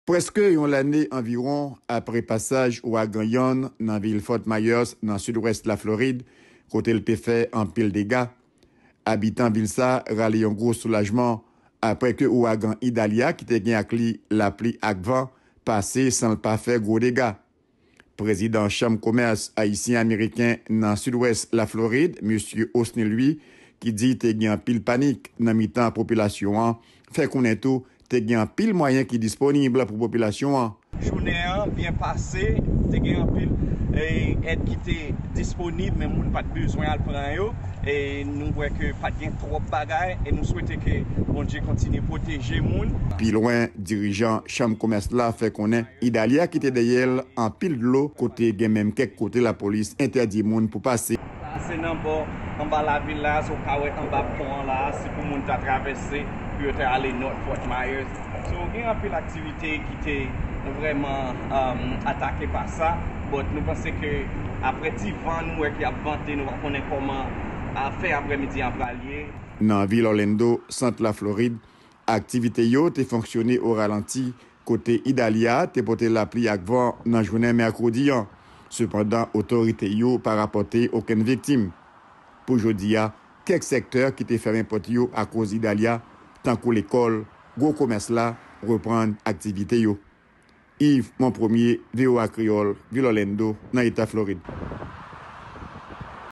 Ayisyen Fort Meyers, Florid Kontan Ouragan Idalia Epaye yo
Ayisyen nan Fort Meyers, Florid rale yon souf apre pasaj Ouragan Idalia paske pa gen gwo dega ki fet. Yve Monpremier gen detay.